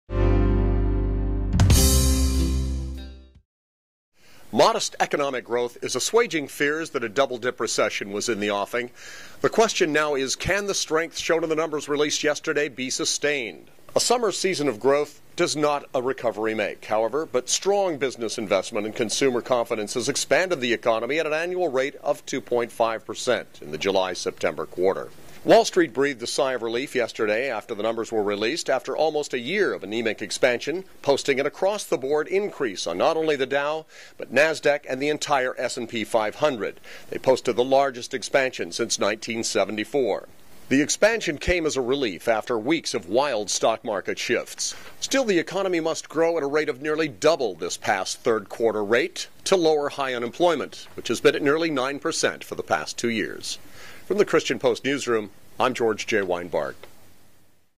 George J. Wienbarg: Third Quarter 2011 Economic Growth Report Spurs a Slim Stock Surge
Third Quarter 2011 growth ads fire to the fizzle of the anemic stock market.